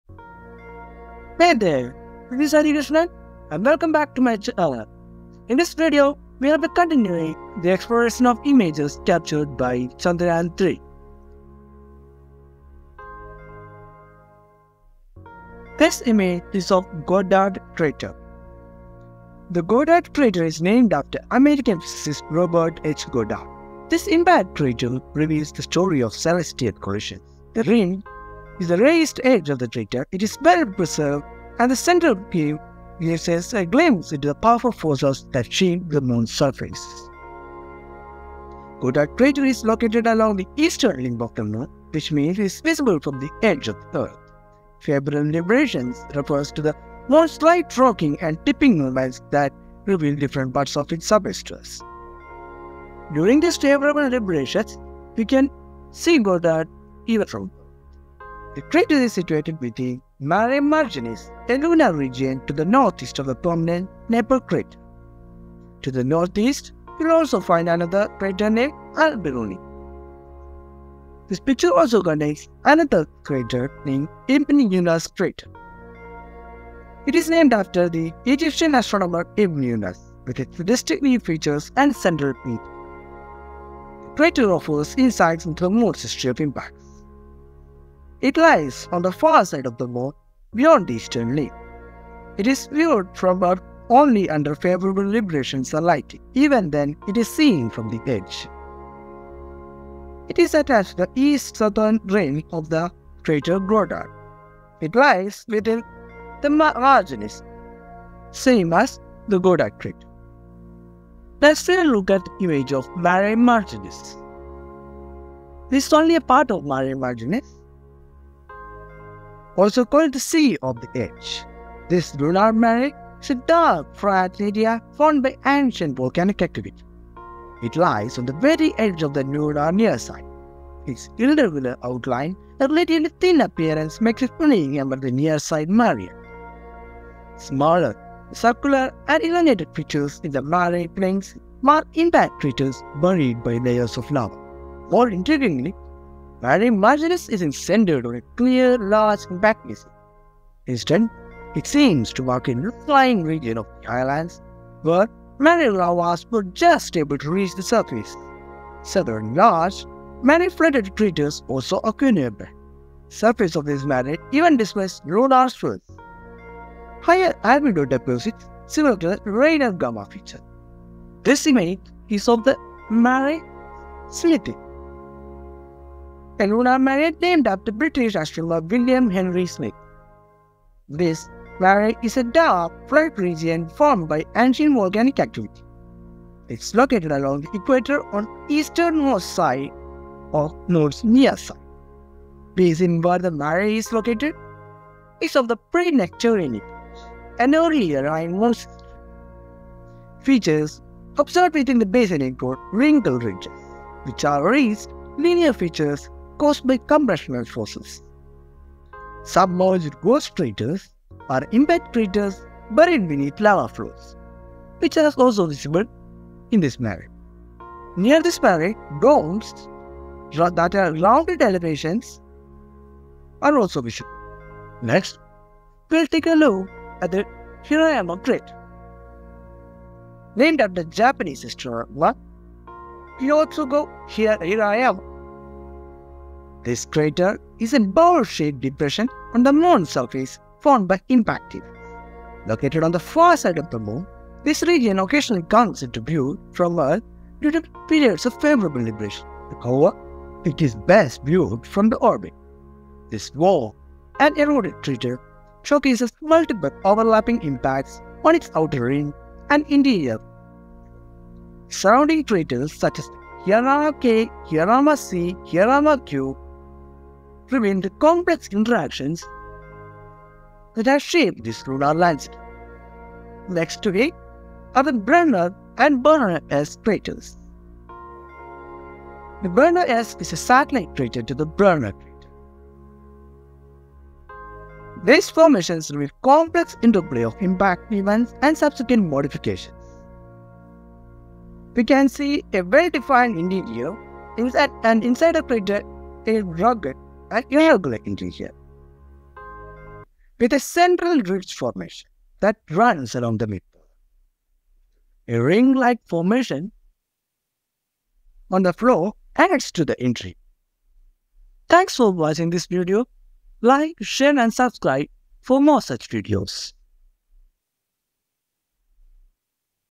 Exploring Lunar Landscapes: Chandrayaan 3's Captivating Moon Photos Part 2
🌙 Embark on an illuminating lunar journey as we decode the captivating lunar photos captured by Chandrayaan 3! Join us in unraveling the enigmas of Goddard, Ibn Yunus, Mare Marginis, Mare Smythii, Hirayama, Brunner, and Brunner S. Let's delve into the lunar landscapes and unveil the hidden stories they hold! 🚀🔍
Image credits:ISRO